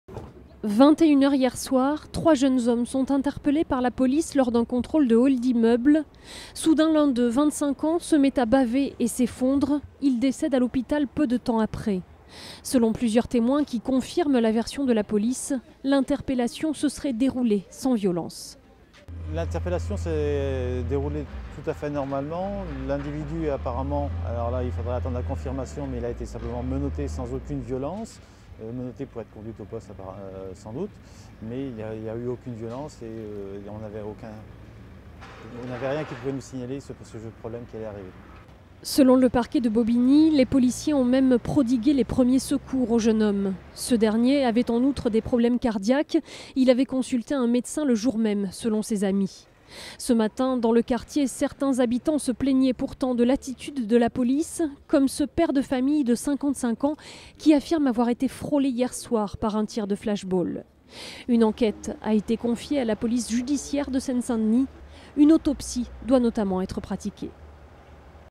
Aulnay : enquête ouverte après la mort d'un homme lors d'un contrôle de police
Le parquet de Bobigny ouvre une enquête après la mort d'un jeune homme ce mardi soir lors d'une interpellation hier à Aulnay-sous-Bois. Le décès pourrait être d'origine cardiaque.